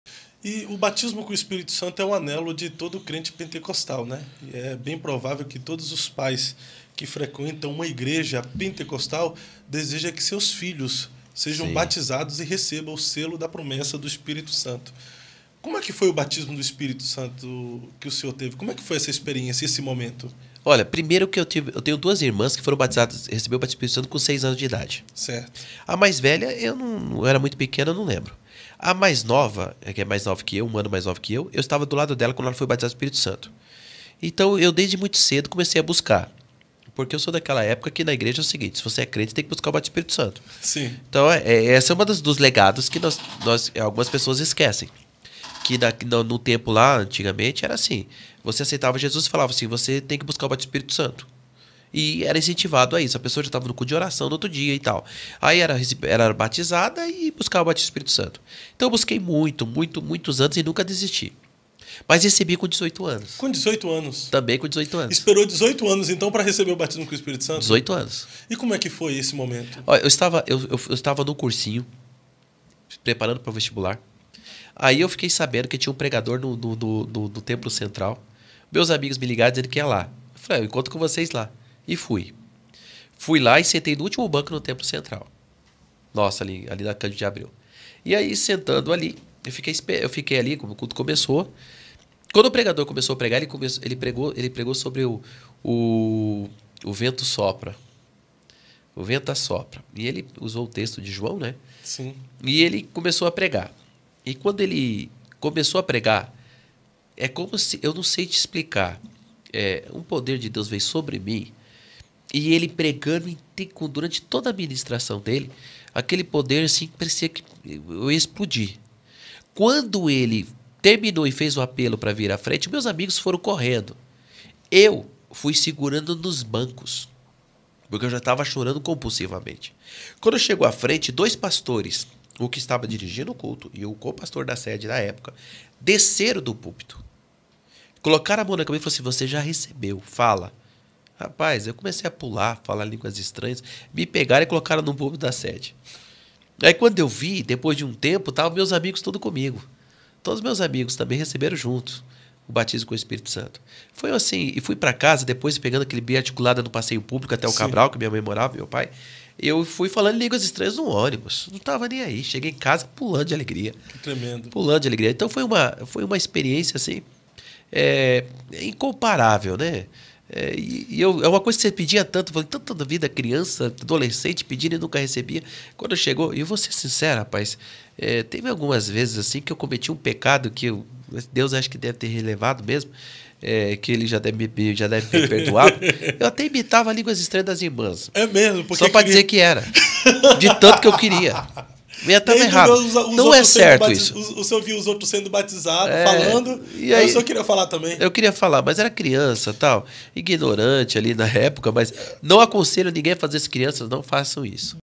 A INEXPLICÁVEL EXPERIÊNCIA DO BATISMO COM ESPÍRITO SANTO
Mais um corte do euacreditopodcast com o pastor Auzias Rodrigues.